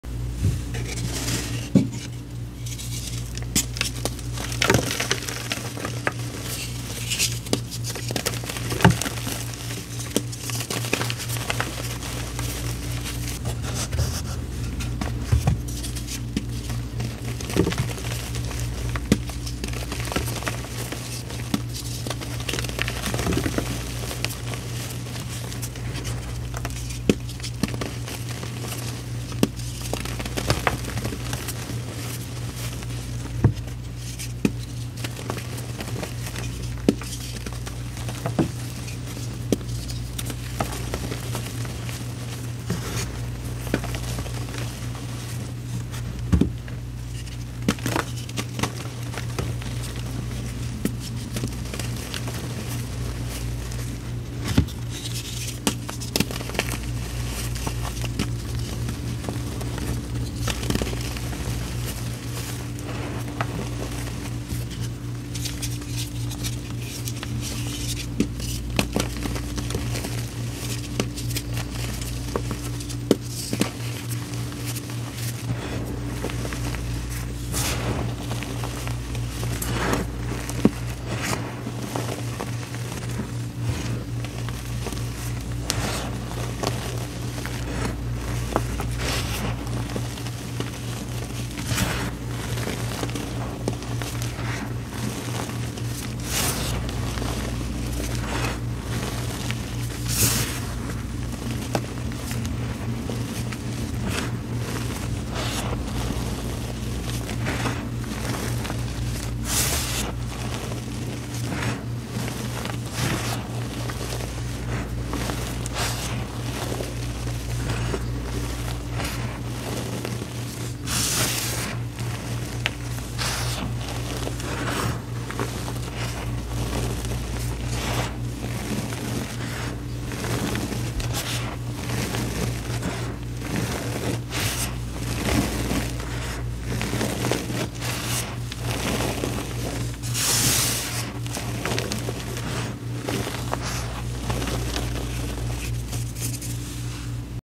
Damp Gymchalk Reform|So satisfying|Sleep Aid| ASMR 💚
This reform wasn't dry all the way but I have to admit I love damp powder it's my favorite to be honest I hope you all enjoy it as much as I did 💚😊

📢 Edits on my YouTube videos allowed only with credit do not remove my watermark or they will be reported🚫